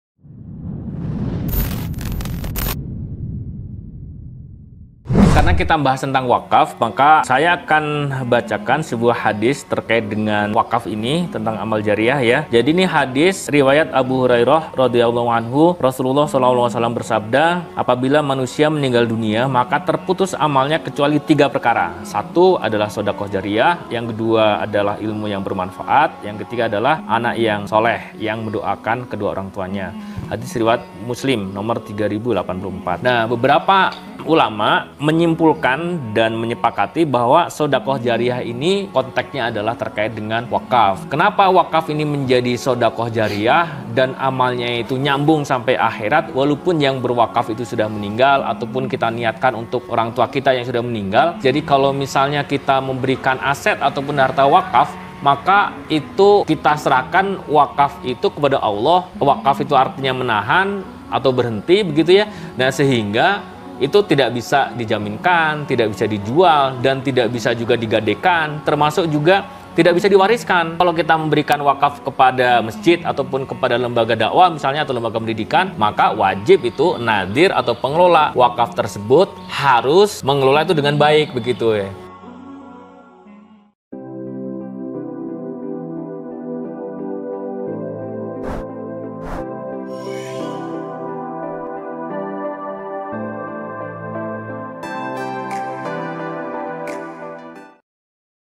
Wakaf itu Amal Jariyah
Sahabatku, wakaf merupakan amal jariyah. Kita bisa mengatas namakan kedua orang tua kita dengan tanah atau harta yang diwakafkan. In syaa Allah akan menjadi amalan jariyah bagi kedua orang tua kita. Mari berwakaf sebagai bentuk rasa syukur kepada Allah.
The waqf is Amal Jariyah
My friend, waqf is a charity. We can name both our parents with waqf land or property. In syaa Allah it will be the practice of jariyah for both our parents. Let waqf as a form of gratitude to Allah.